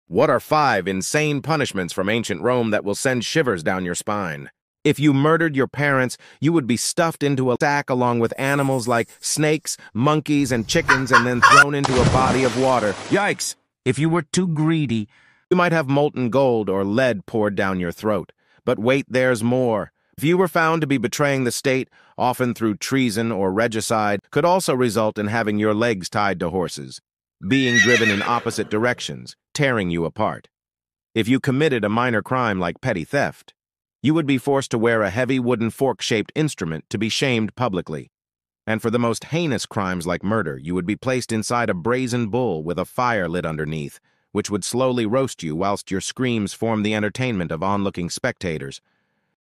5 INSANE Punishments from Ancient Rome😱that will send shivers
From fire to flood, here are some of the worst and weirdest punishments from Ancient Rome that will send shivers down your spine

Don't miss out on more absurd historical facts and insights! ⌛🏺